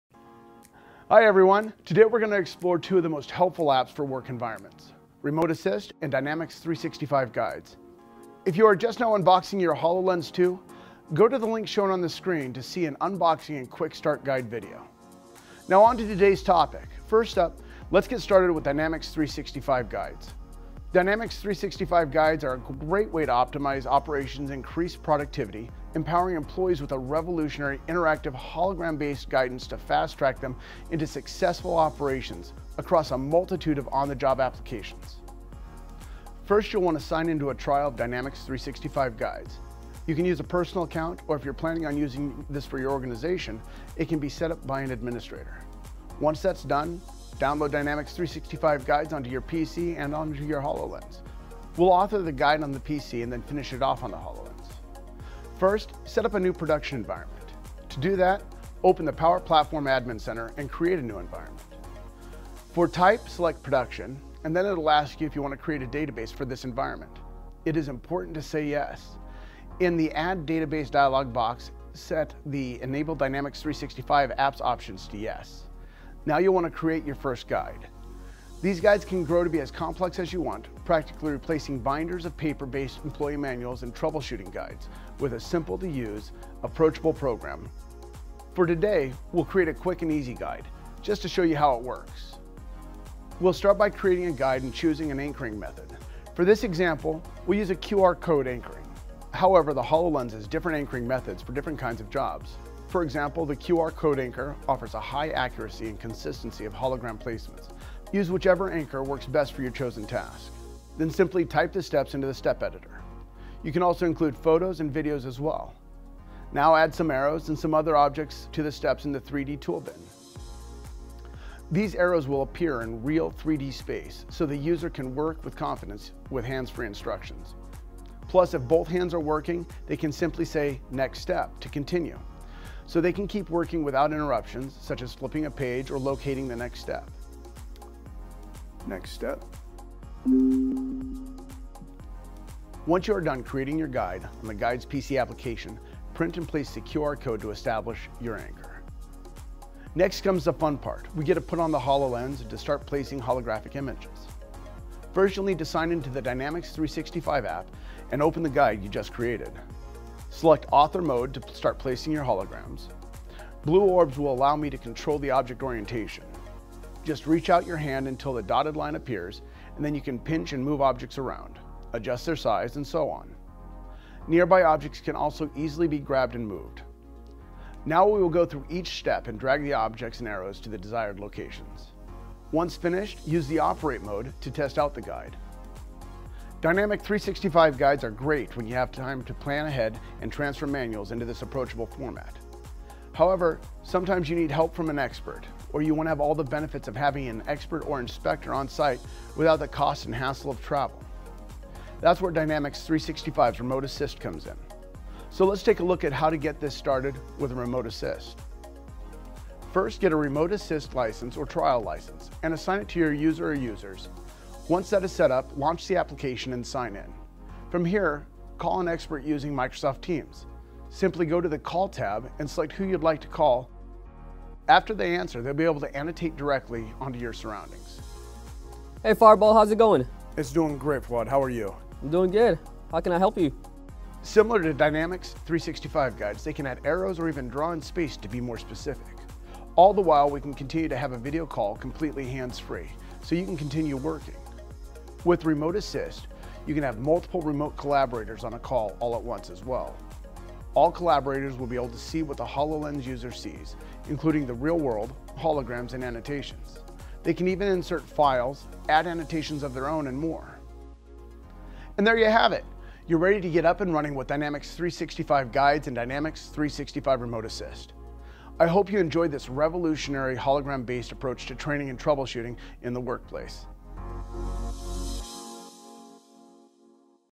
HoloLens: Remote assist and guides
Fireball Brady dives into the details of how to get started with two of the HoloLens’ most popular industrial apps: 365 Remote Assist and Guides.